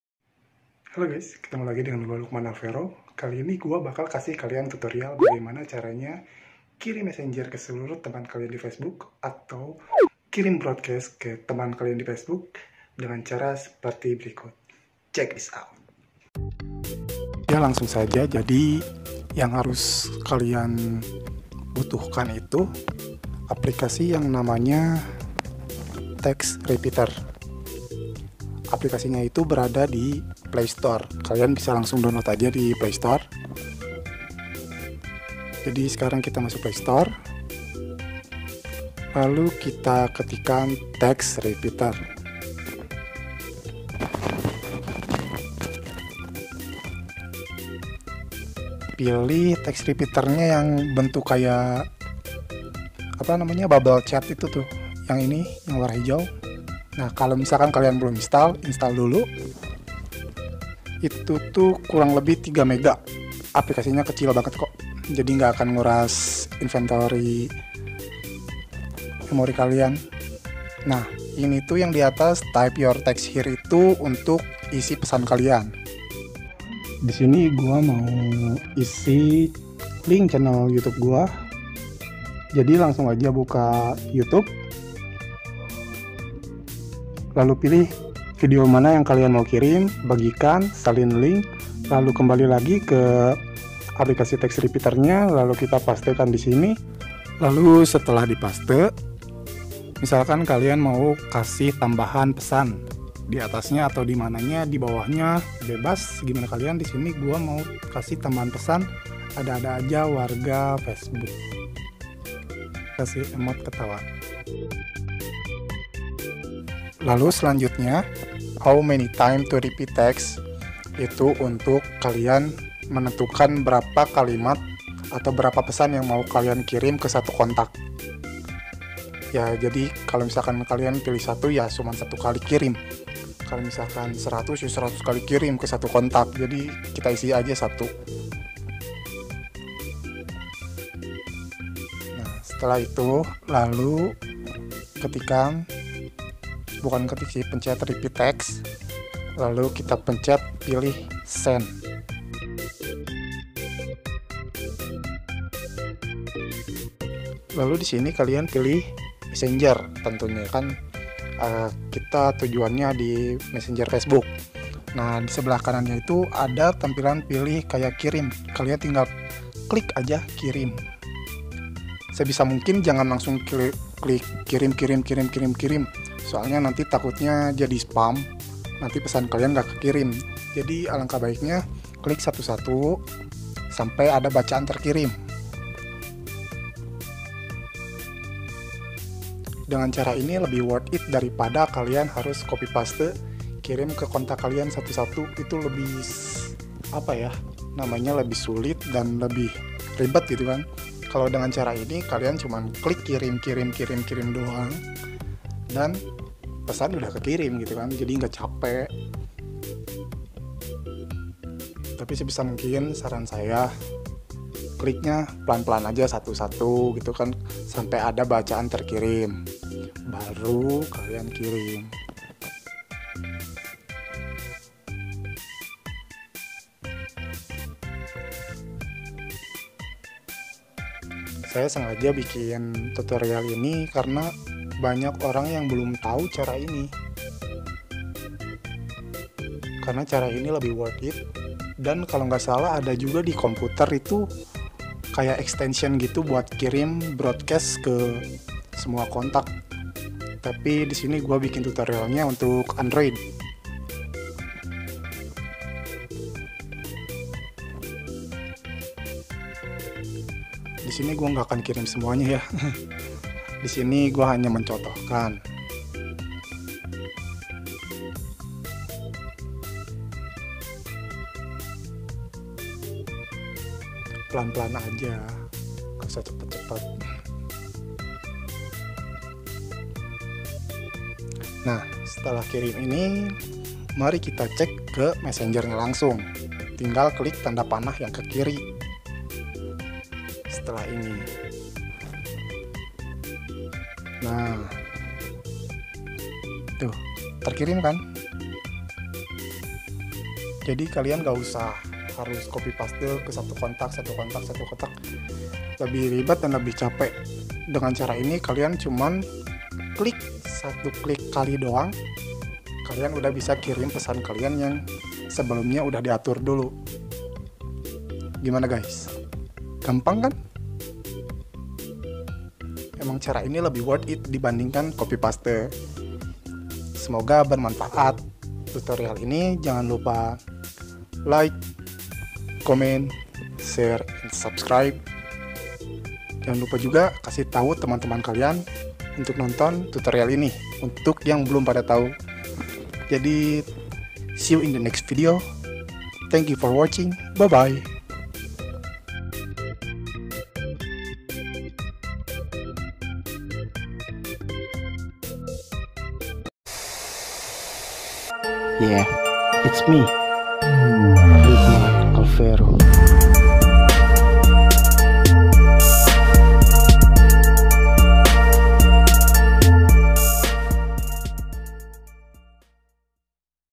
CARA KIRIM PESAN KE SEMUA TEMAN DI FACEBOOK - TEXT REPEATER
CARA KIRIM PESAN KE SEMUA TEMAN DI FACEBOOK

Aplikasi Yang Dibutuhkan :
- Text Repeater (Tersedia di Google Play)
- Facebook Messenger

Langkah-langkah :
1. Kalian install "Text Repeater" di playstore dengan besar file 3MB.
2. Buka aplikasi "Text Repeater" lalu pada kolom "Type Your Text Here" isilah pesan yang kalian mau BC (Broadcast Pesan).
3. "How Many Times to Repeat Text" kalian ubah yang asalnya 100 menjadi 1 (sesuai kebutuhan kalian sih sebenernya)
4. Pada tutorial di atas saya promosi link video youtube terbaru saya, jadi saya pergi ke youtube lalu saya salin url video terbarunya lalu paste pada kolom "Type Your Text Here".
5. Jika ingin menambahkan pesan lagi tulis lagi aja bro.
6. KLIK button "REPEAT TEXT" maka akan keluar button dengan bertuliskan COPY, SEND & RESET
7. KLIK button "SEND" maka akan muncul pilihan kalian akan share ke social media mana, karena berhubung ini tutorial "BROADCAST PESAN FACEBOOK" maka pilih "FACEBOOK MESSENGER"
8. KLIK button yang bertuliskan "KIRIM" untuk kalian kirim pesan yang sudah kalian buat sebelumnya.
9. Sebisa mungkin jangan kalian terlalu cepat klik tombol "KIRIM" karena itu akan menimbulkan spam dan kalian tidak akan bisa kirim pesan.
10. CEK MESSENGER KALIAN